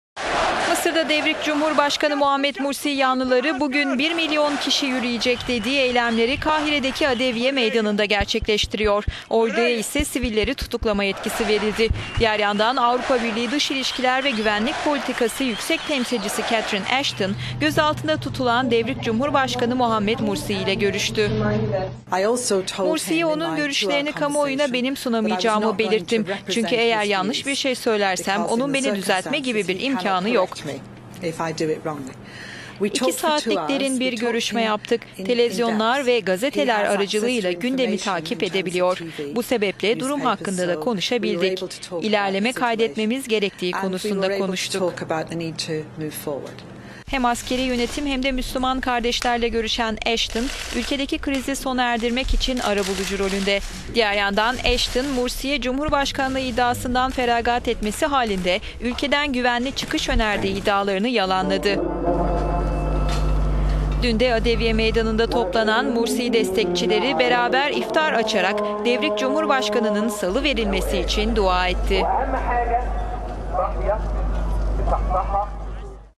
Ashton Mursi'ye güvenli çıkış önerdiği iddialarını yalanladı
Mısır'da devrik cumhurbaşkanı Muhammed Mursi yanlıları bugün 'bir milyon kişi yürüyecek'...

Mısır'da devrik cumhurbaşkanı Muhammed Mursi yanlıları bugün 'bir milyon kişi yürüyecek' dediği eylemleri Kahire'deki Adeviye Meydanı'nda gerçekleştiriyor. Orduya ise sivilleri tutuklama yetkisi verildi.

Diğer yandan Avrupa Birliği Dış İlişkiler ve Güvenlik Politikası Yüksek Temsilcisi Catherine Ashton, gözaltında tutulan devrik Cumhurbaşkanı Muhammed Mursi ile görüştü:

"Mursi'ye onun görüşlerini kamuoyuna benim sunamayacağımı belirttim çünkü eğer yanlış birşey söylersem onun beni düzeltme gibi bir imkanı yok. İki saatlik derin bir görüşme yaptık. Televizyonlar ve gazeteler aracılığıyla gündemi takip edebiliyor. Bu sebeple durum hakkında da konuşabildik. İlerleme kaydetmemiz gerektiği konusunda konuştuk."

Hem askeri yönetim hem de Müslüman Kardeşler ile görüşen Ashton, ülkedeki krizi sona erdirmek için arabulucu rolünde. Diğer yandan Ashton, Mursi'ye cumhurbaşkanlığı iddiasından feragat etmesi halinde ülkeden güvenli çıkış önerdiği iddialarını yalanladı.